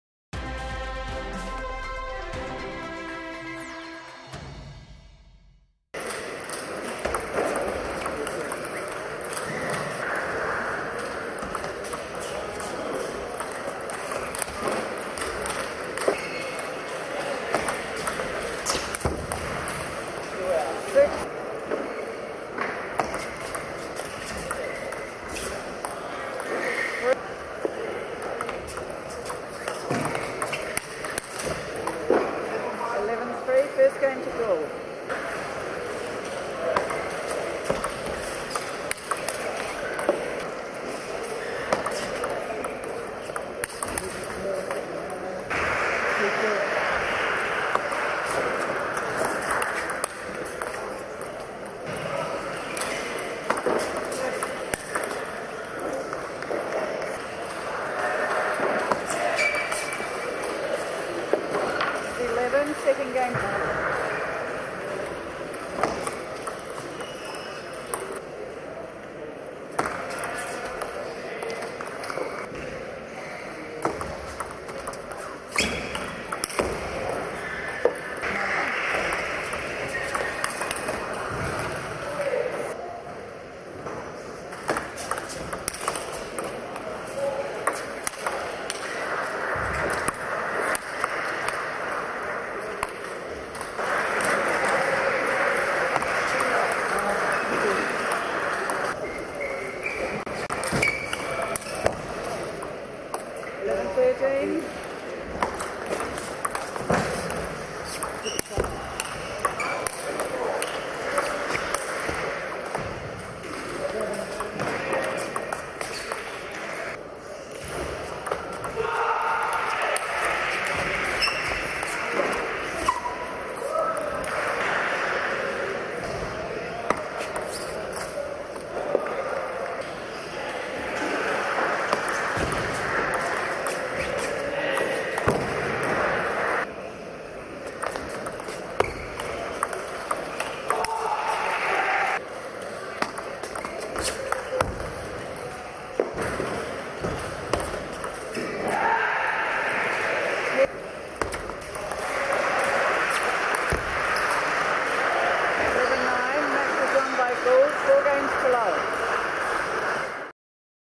2016 Australian Open Highlights: Benjamin Gould vs Huang Sida (Qual)
Review all the highlights from the Benjamin Gould vs Huang Sida (Qual) from the 2016 Australian Open